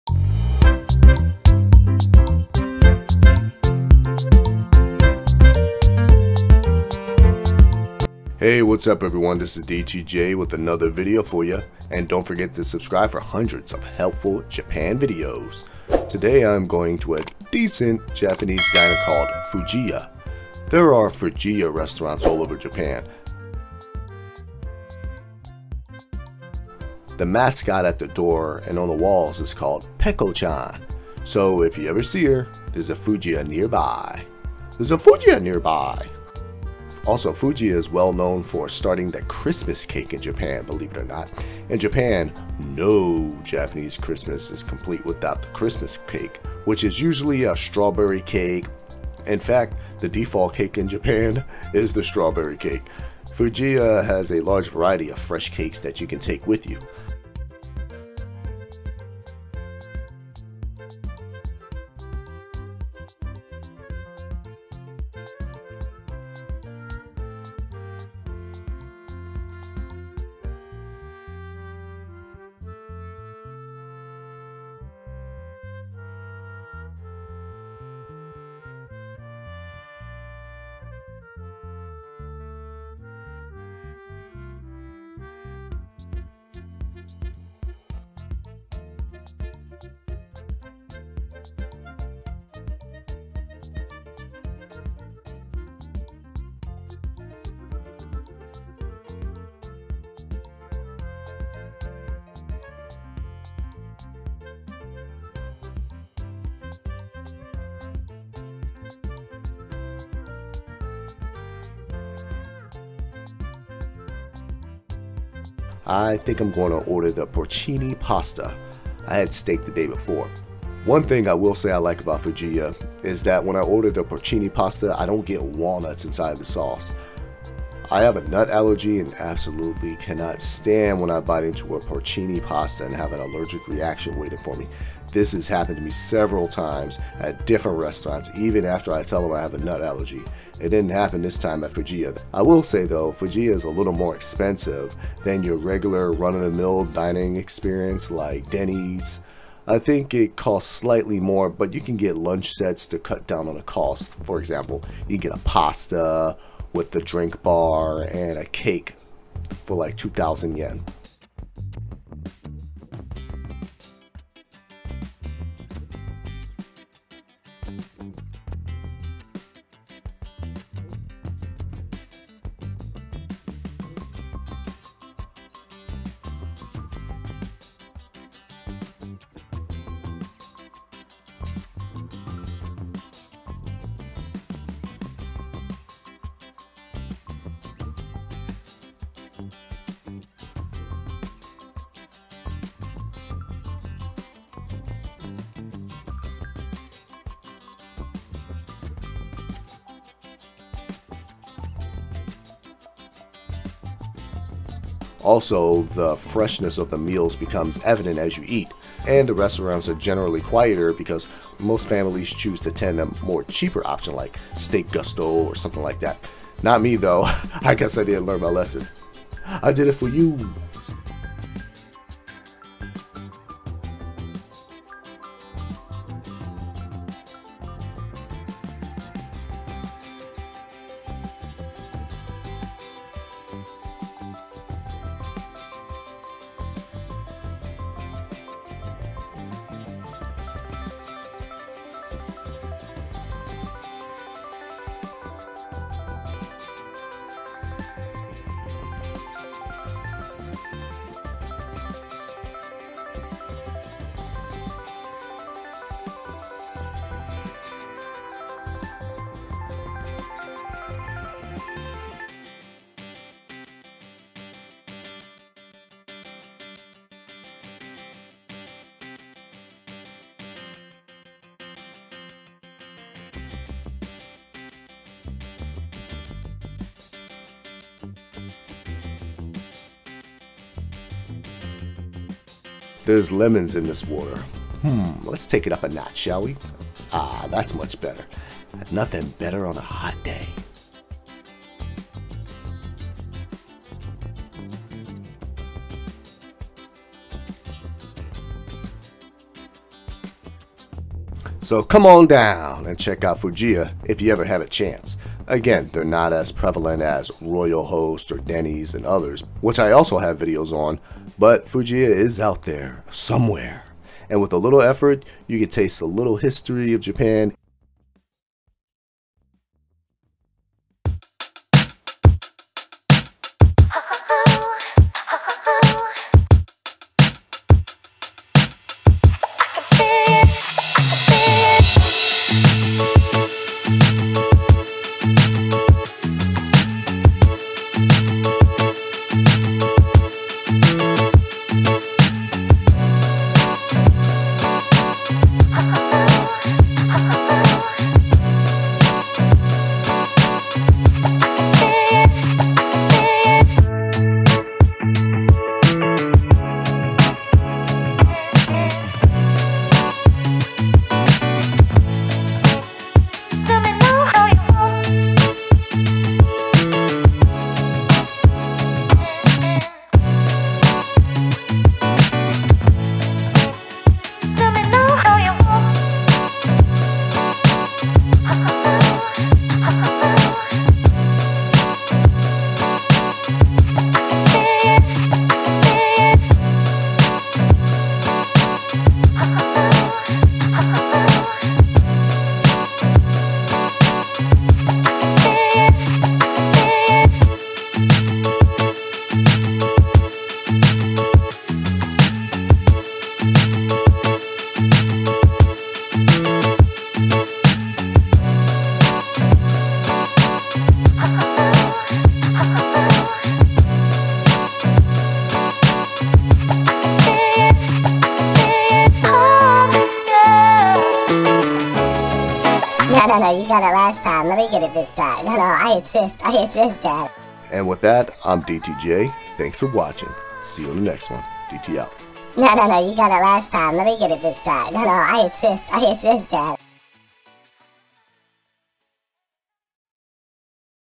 These shortcakes in Japan are so loaded with strawberries I damn near choked
What started out as a confectionary spawned into a diner chain rich with delicious cakes . This restaurant has just about everything else you would find at other restaurants but with lots of cakes ! You can’t say no to cakes, and Fujiya has it in spades . See what’s on the menu and learn what you can expect when you come here .